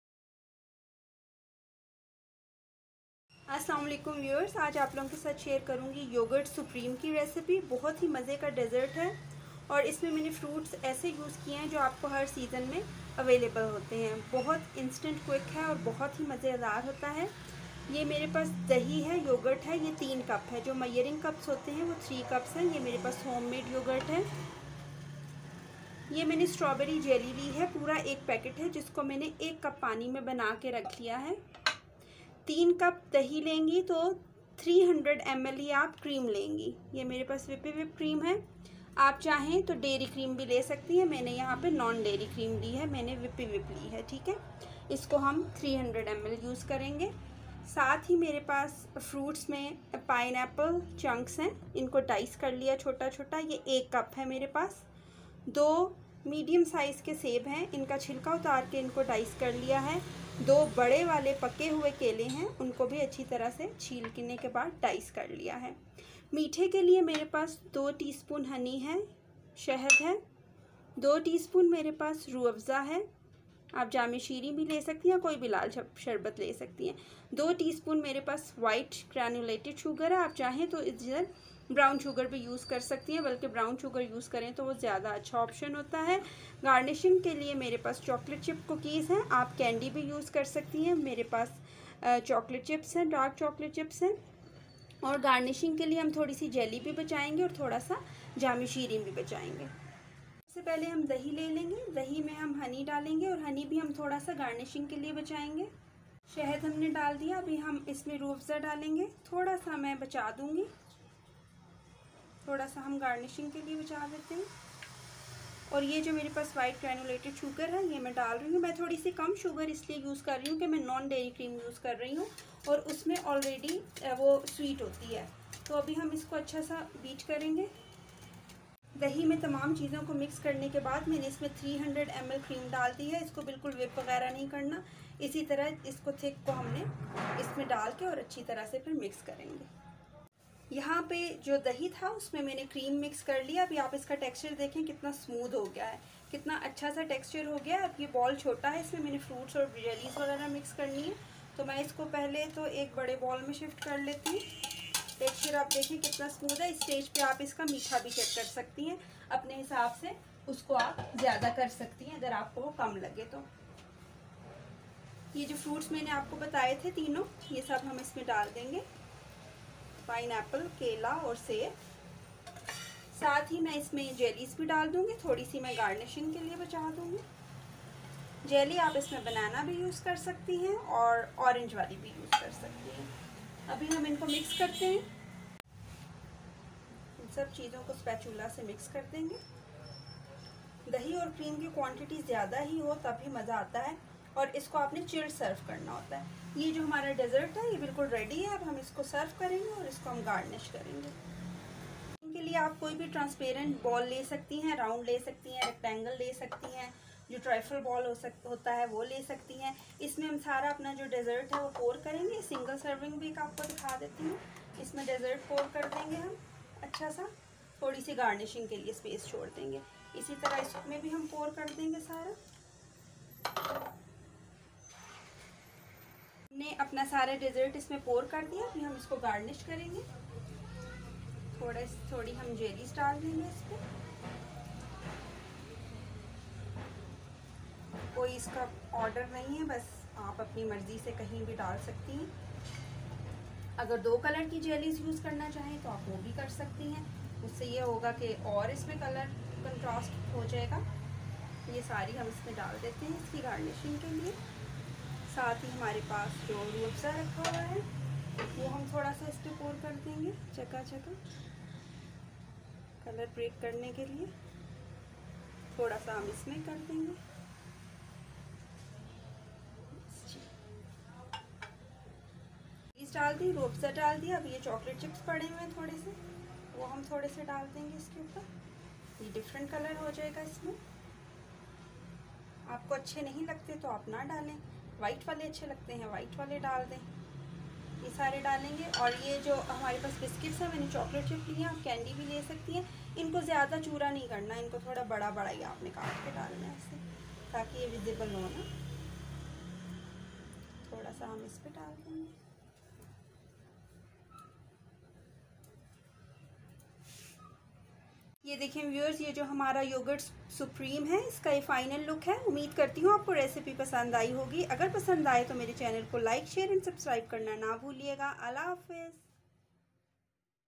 Yogurt supreme ll yogurt delight ll creamy yogurt delight ll yogurt cream trifle ll daawat dezzert
How to make yogurt supreme
Ingredients
1. Yogurt (3 cups)
2.cream (300 ml)
3.rooh afza(2 tsp)
4.honey(2 tsp)
5.sugar(2 tsp)
6.jelly (1 packet)
7.pineapple(1 cup)
8.apples(2)
9.bananas (2)
10.chocolate chips(for garnishing)
11.candy biscuits (for garnishing)
Method
1.make jelly in 1 cup.keep aside
2. Peel n dice all fruits.keep aside
3.in a mixing bowl, mix yogurt with honey,rooh afza and sugar.
4.add cream n mix well to form a good smoothy texture
5.add fruits n jellies n mix
6.pour in serving bowl
7.grnish with jellies, chocolate chip and candy
8.serve chilled